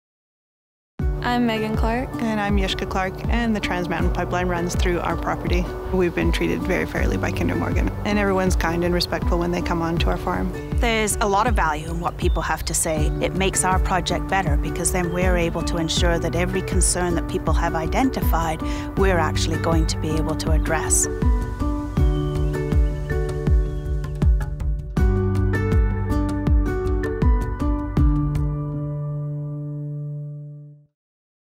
Working Together - The People Behind The Pipeline
Meet Meghan and Jushka Clarke and Lizette Parsons Bell as they talk about the employees at Kinder Morgan Canada who work with people in our pipeline communities to address questions, concerns and feedback.